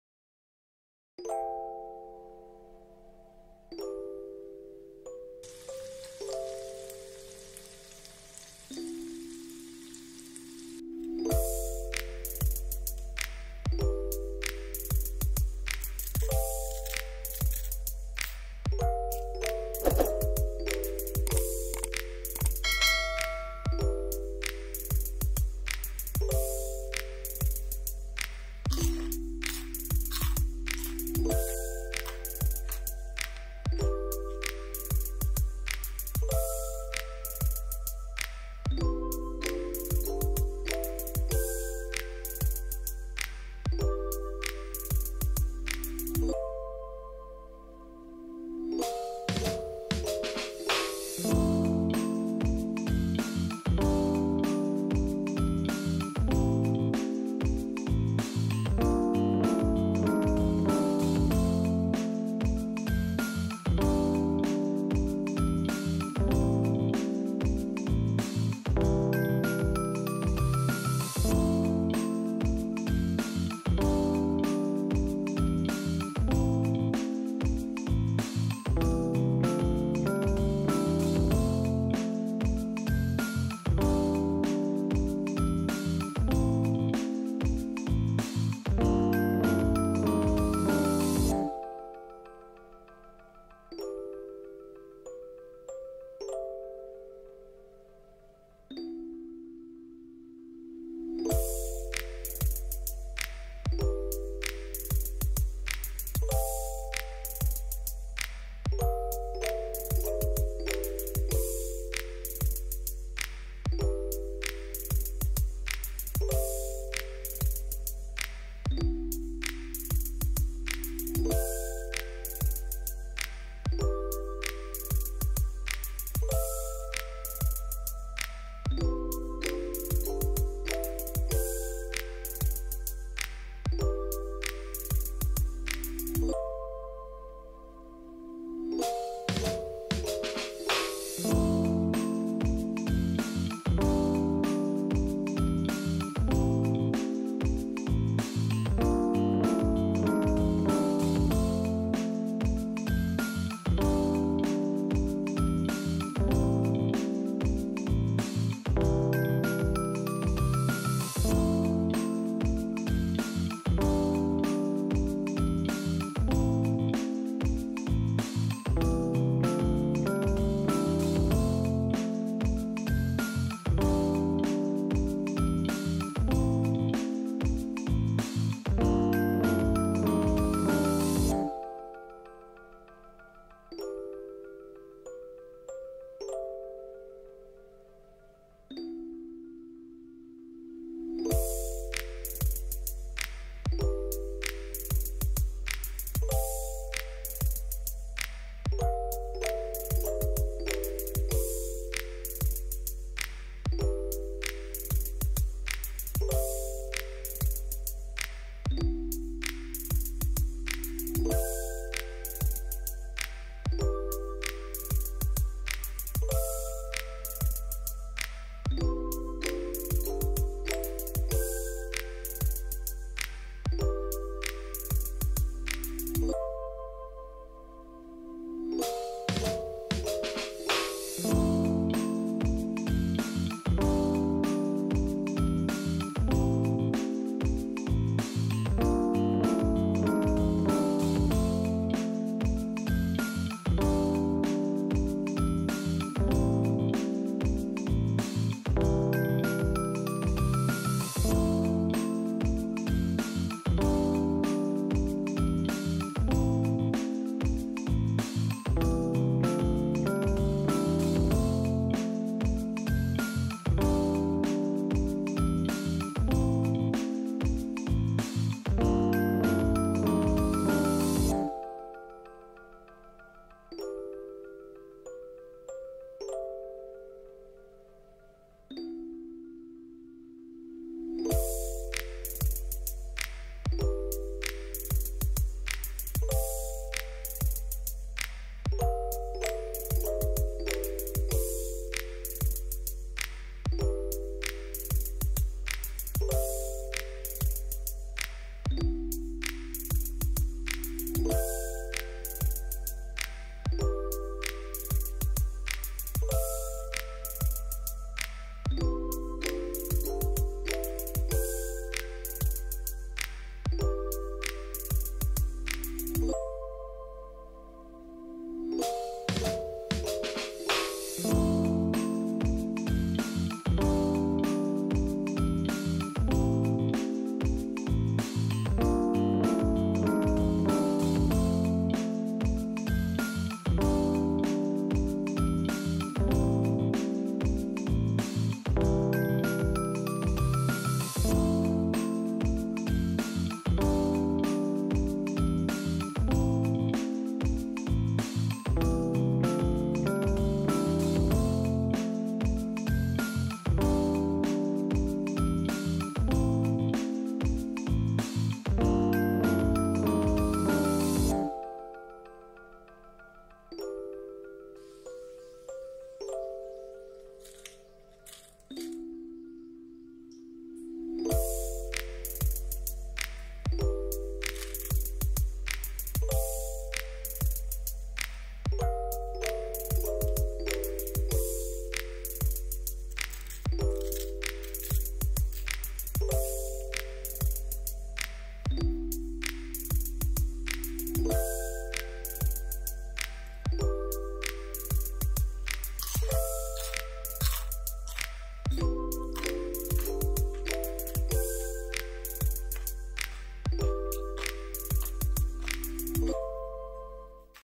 Irresistible Crispy Fried Tofu. Perfect Finger Food Recipe
1 cup / cwn = 250 ml

3 firm tofu / 3 keping tauhu keras
half cup corn starch flour / separuh cwn tepung jagung
70 ml water / 70 ml air
half tsp crushed black pepper / separuh sk serbuk lada hitam
half tsp onion powder / separuh sk serbuk bawang merah
salt / garam
1 cup breadcrumbs / 1 cwn serpihan roti

Sauce / Sos

2 tbs tomato ketchup / 2 sb sos tomato
2 tbs soya sauce / 2 sb kicap masin
1 tbs chilli flakes / 1 sb serpihan cili
1 tsp sesame oil / 1 sk minyak bijan
i tsp sweet soy sauce / 1 sk kicap manis
3 cloves garlic / 3 ulas bawang putih
half tsp garlic powder / separuh sk sebuk bawang putih
1 tsp sesame seeds / 1 sk bijan

Fusion Cook Space
Happy Trying 😊